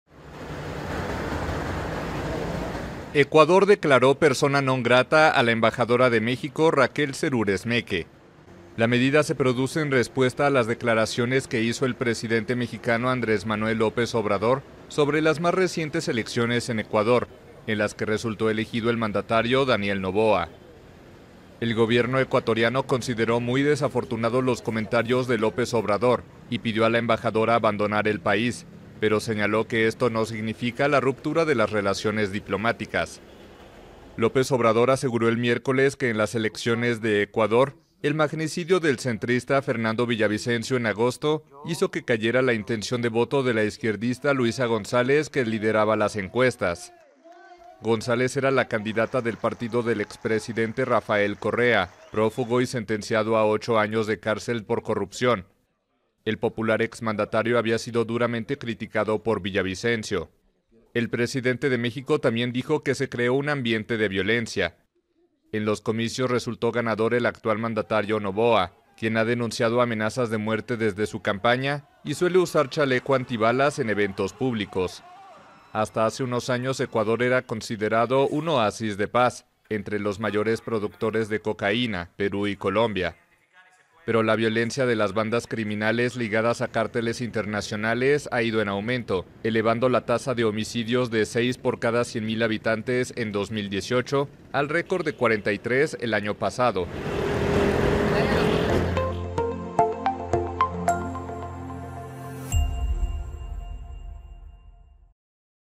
Ecuador declara "persona non grata" a embajadora mexicana por dichos de López Obrador
Ecuador pidió el jueves a la embajadora de México en Quito abandonar el país al declararla "persona non grata", tras las críticas del presidente Andrés Manuel López Obrador sobre las últimas elecciones ecuatorianas en las que fue asesinado un candidato.